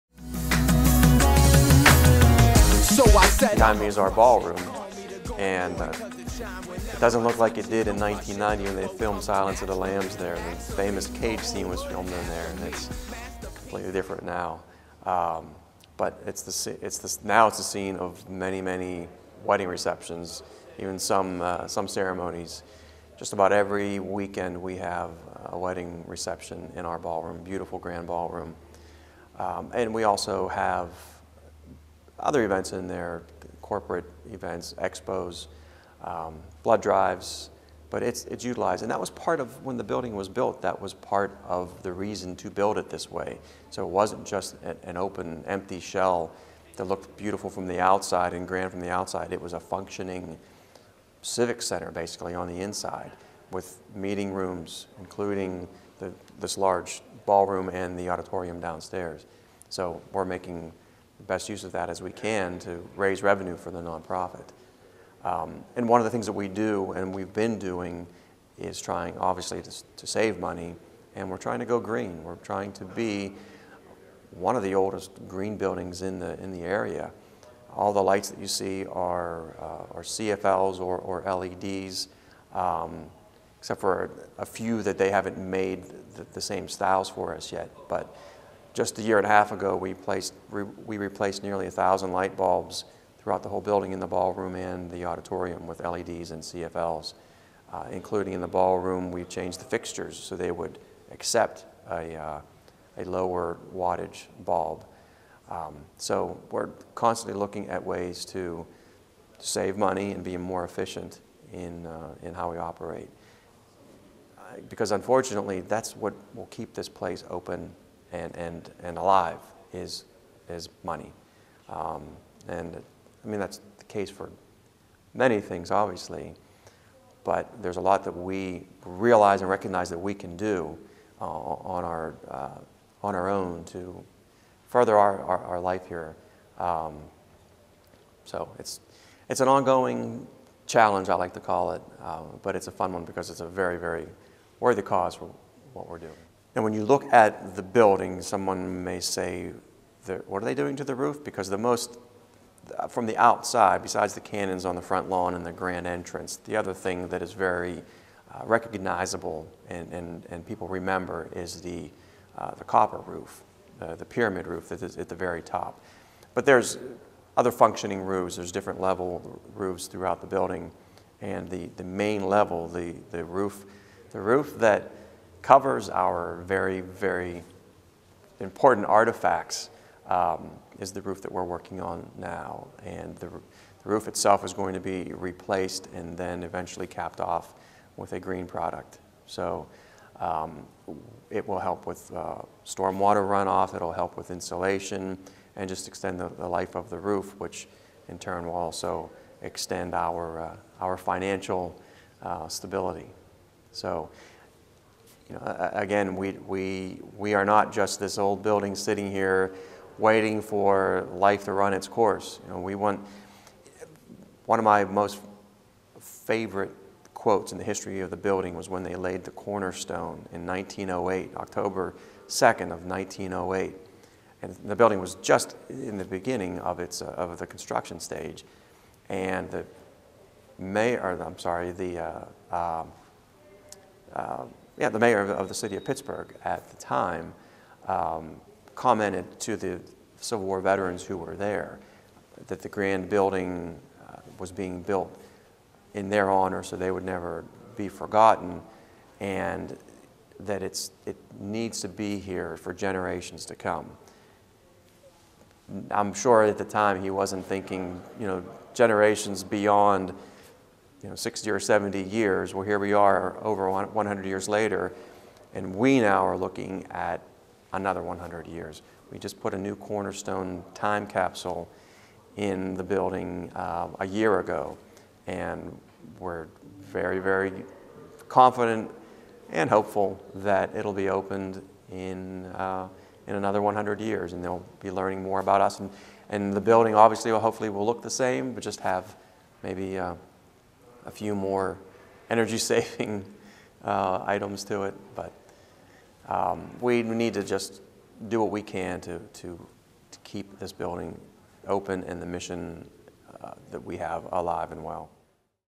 Unsung Uncut: Ballroom and Greening Up
President and CEO of Soldiers and Sailors John McCabe tells us about the ballroom, famous for it's roll in Silence of the Lambs, and tells more about the efforts to make the building green and keep the place self sustaining.